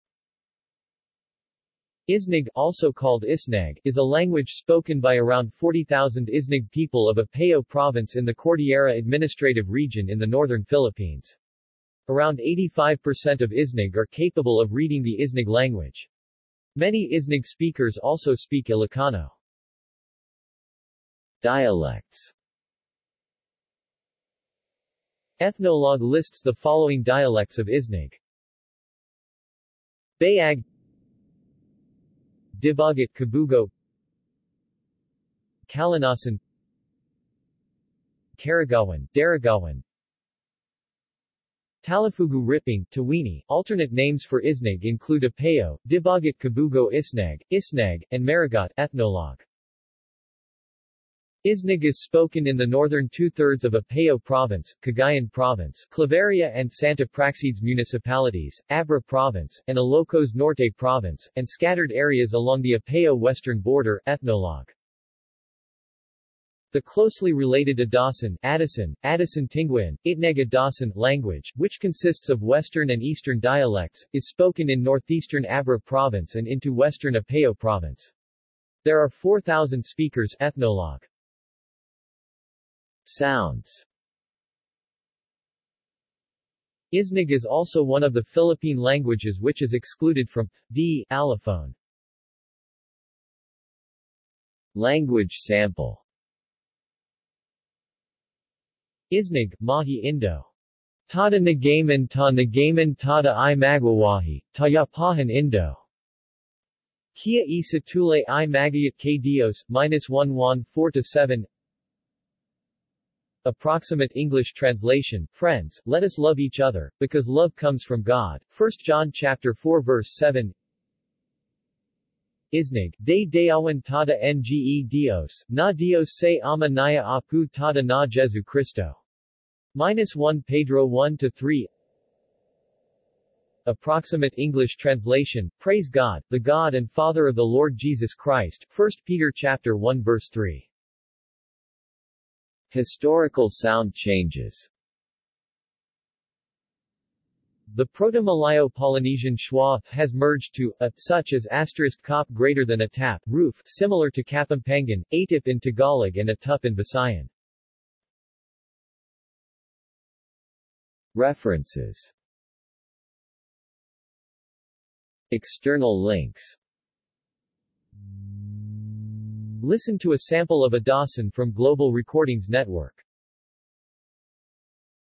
Isnag language | Wikipedia audio article
This is an audio version of the Wikipedia Article:
Isnag language

SUMMARY
Isnag (also called Isneg) is a language spoken by around 40,000 Isnag people of Apayao Province in the Cordillera Administrative Region in the northern Philippines. Around 85% of Isnag are capable of reading the Isnag language. Many Isnag speakers also speak Ilocano.